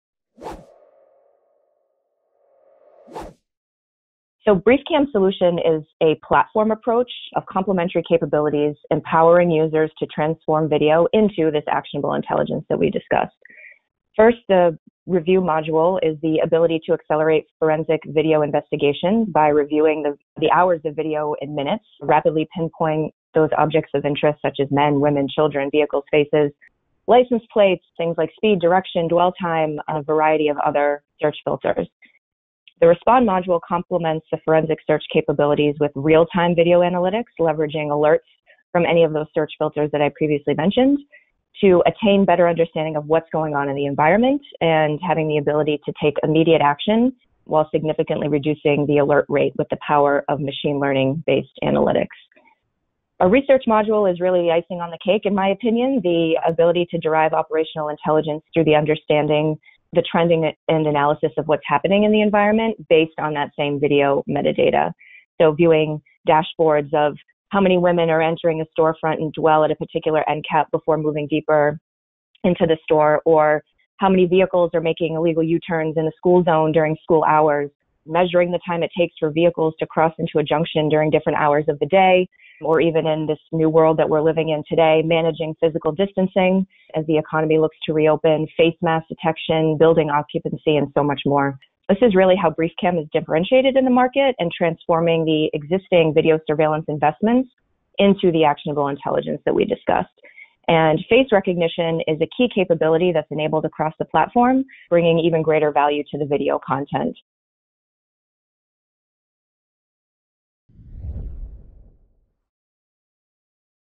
BriefCam Video Content Analytics Platform Overview
This quick overview of the BriefCam® comprehensive Video Content Analytics Platform demonstrates how video can be made searchable, actionable and quantifiable for driving exponential value from existing surveillance camera investments. BriefCam offers a robust portfolio of mission-critical video analytics for:
1. Accelerating investigations with the REVIEW module
2. Attaining situational awareness with the RESPOND module
3. Deriving operational intelligence with the RESEARCH module
This video will walk through the different components of BriefCam's solution, demonstrating our video analysis capabilities fully integrated across the platform, such as face recognition, and how this sets our solution apart.